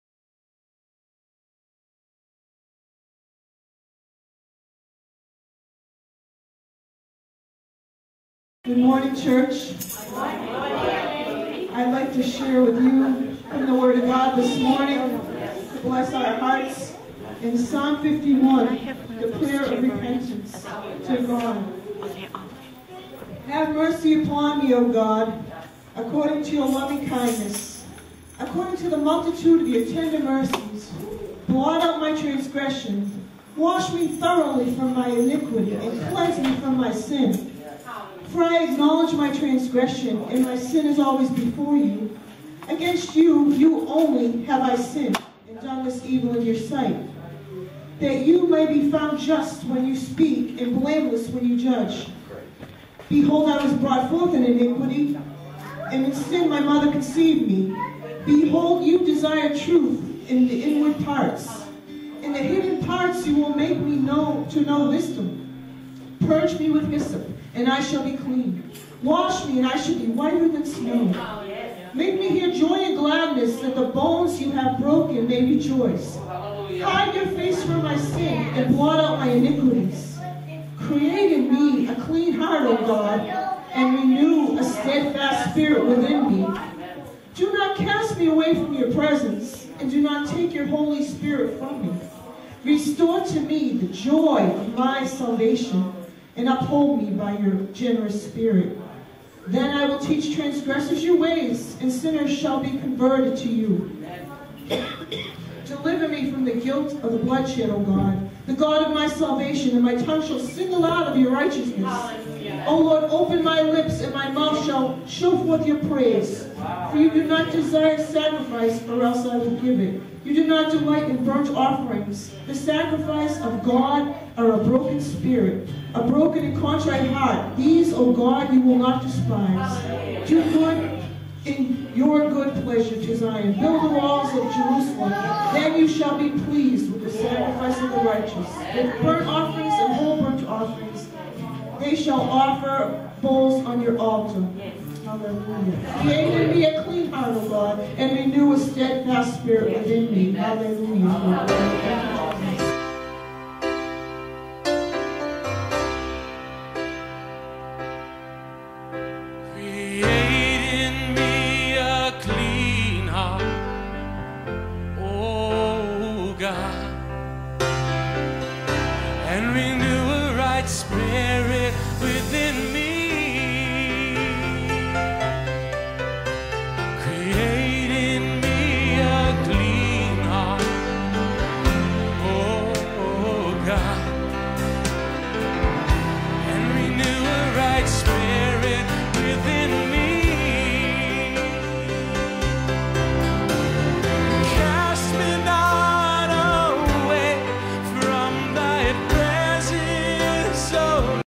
Scripture Reading: Psalm 51
Sunday Worship (March 8, 2015) Judy Pereira